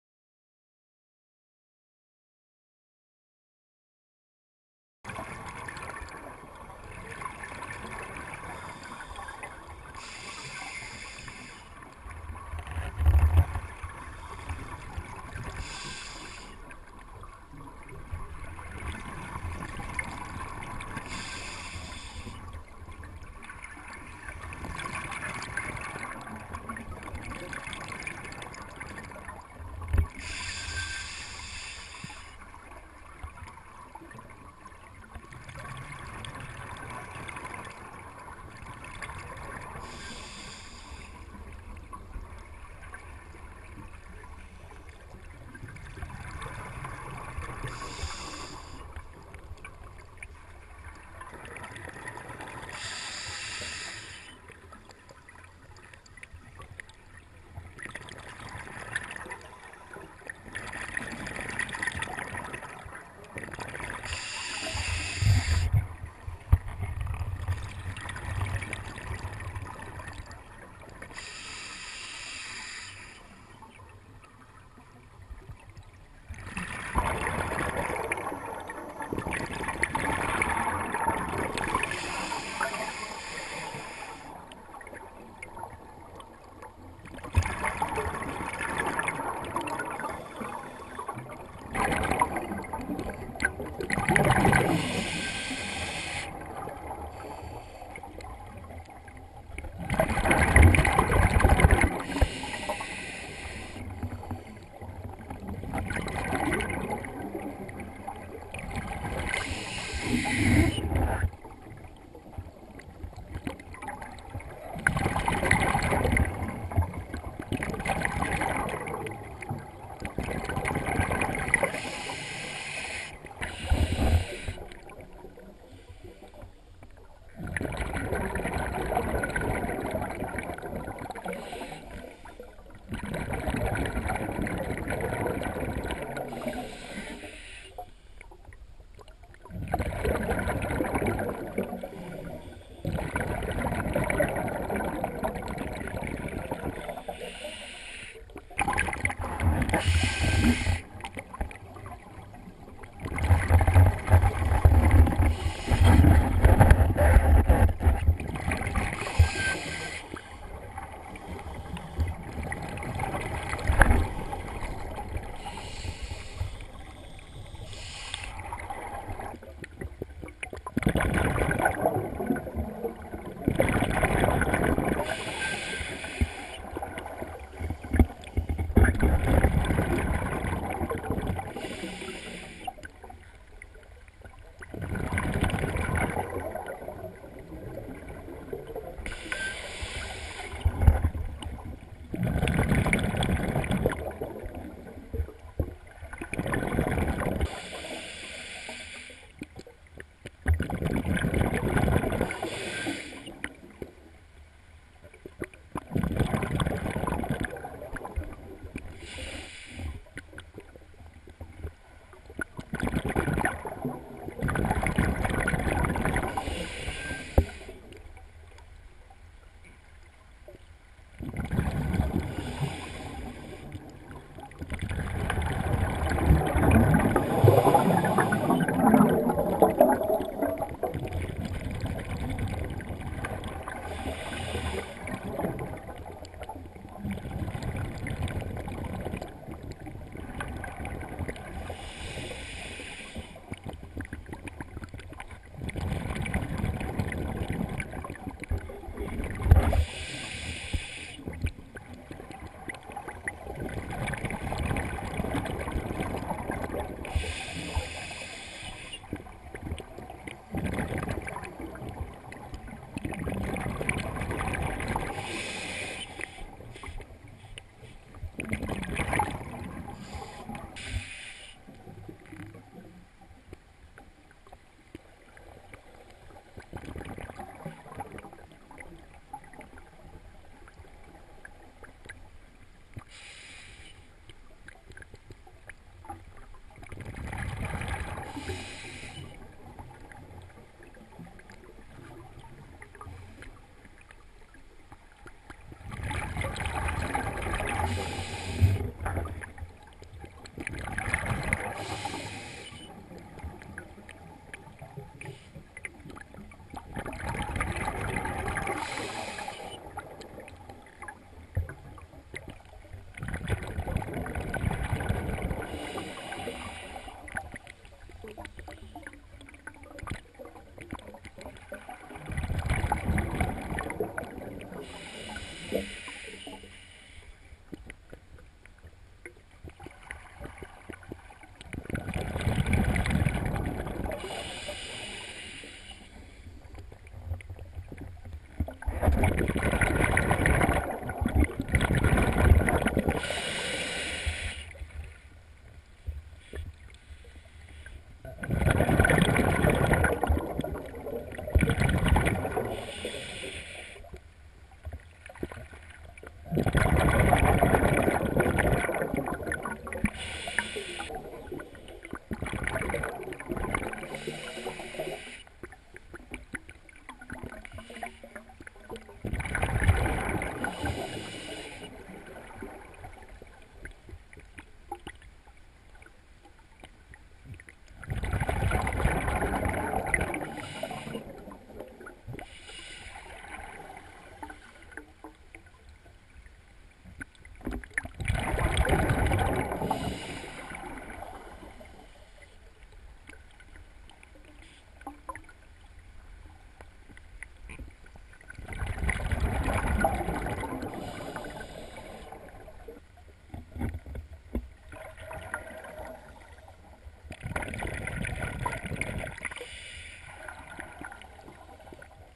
Underwater forest
Few hundred perches, a pike and an eel living in an underwater forest.